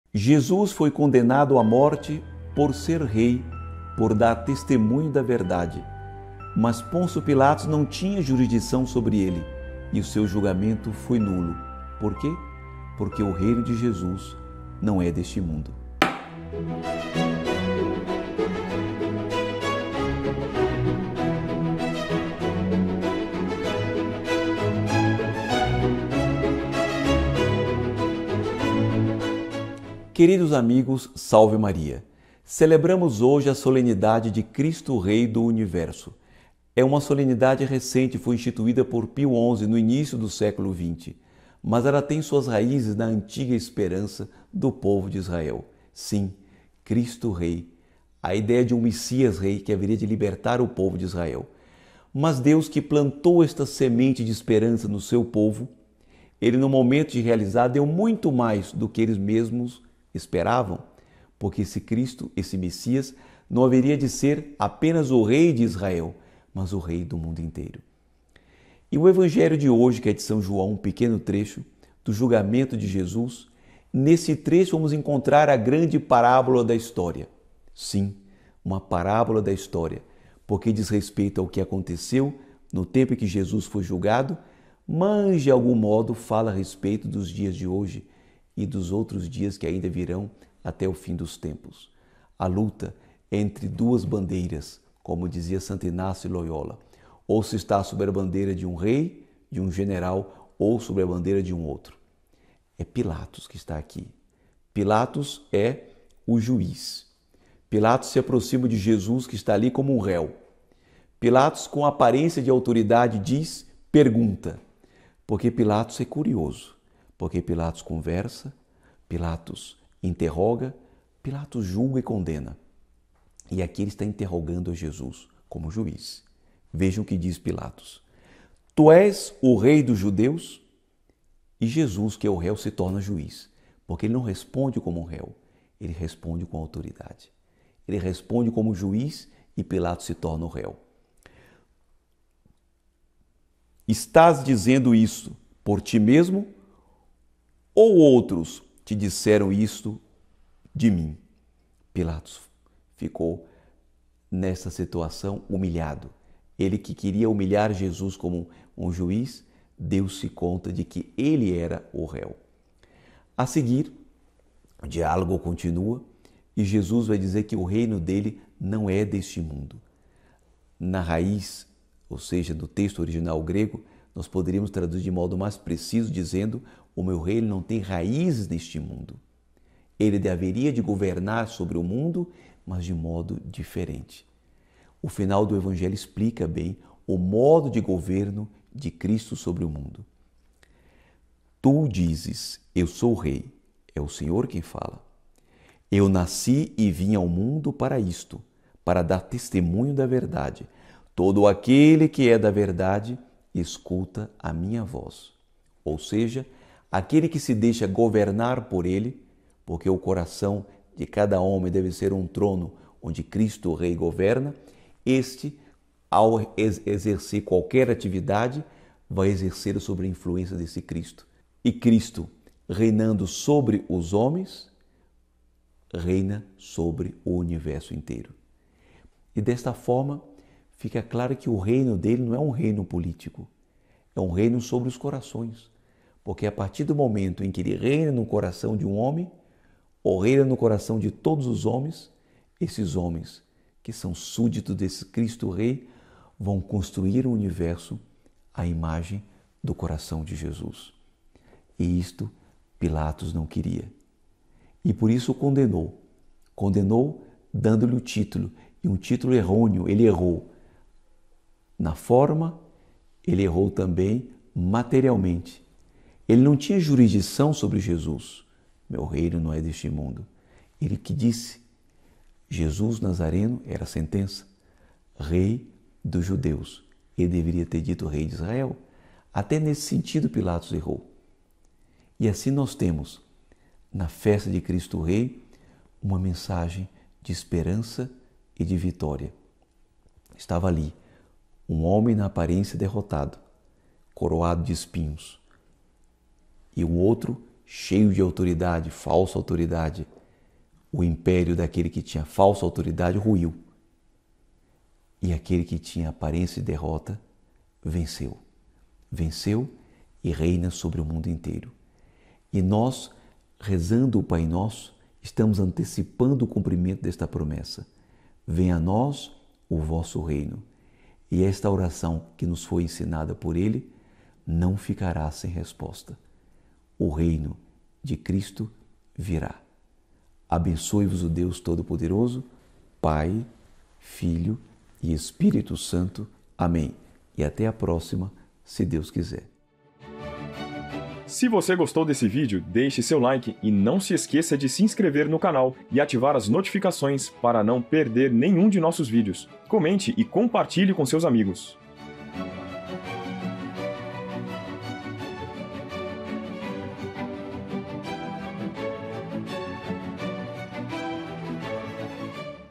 Por que Jesus foi condenado? Porque Ele é Rei! | Liturgia Diária ( 21 nov. 2021)
Solenidade de Jesus Cristo, Rei do Universo
O último domingo do calendário litúrgico a Igreja reserva para a comemoração da grandiosa solenidade de Cristo Rei do Universo.

Cristo de fato é o Rei do Universo e também dos nossos corações. Ele possui uma autoridade absoluta sobre todas as criaturas e já muito antes de sua Encarnação, quando se encontrava no seio do Padre Eterno, ouviu estas palavras:

“Tu és meu Filho, eu hoje te gerei. Pede- me; dar-te-ei por herança todas as nações; tu possuirás os confins do mundo, tu governarás com cetro de ferro” (Sl 2, 7-9).

A festa, anual, de “Cristo-Rei” dá-nos a mais viva esperança de acelerarmos a tão desejada volta da humanidade a seu Salvador amantíssimo (…) É uma festa, anualmente celebrada por todos os povos em homenagem a Cristo-Rei.

Com estas palavras, o Papa Pio XI instituiu esta festa:
Portanto, em virtude de Nossa autoridade apostólica, instituímos a festa de “Nosso Senhor Jesus Cristo Rei”, mandando que seja celebrada cada ano, no mundo inteiro, no último domingo de outubro (…) porque ele, em certo modo, encerra o ciclo do ano litúrgico. Destarte, os mistérios da vida de Jesus Cristo, comemorados no decorrer do ano que finda, terão na solenidade de “Cristo-Rei” seu como termo e coroa.

Evangelho - Jo 18,33b-37

Naquele tempo, Pilatos chamou Jesus e perguntou-lhe: “Tu és o rei dos judeus?” Jesus respondeu: “Estás dizendo isto por ti mesmo ou outros te disseram isto de mim?”

Pilatos falou: “Por acaso sou judeu? O teu povo e os sumos sacerdotes te entregaram a mim. Que fizeste?”

Jesus respondeu: “O meu reino não é deste mundo. Se o meu reino fosse deste mundo, os meus guardas lutariam para que eu não fosse entregue aos judeus. Mas o meu reino não é daqui”.

Pilatos disse a Jesus: “Então tu és rei?”

Jesus respondeu: “Tu o dizes: eu sou rei. Eu nasci e vim ao mundo para isto: para dar testemunho da verdade. Todo aquele que é da verdade escuta a minha voz”.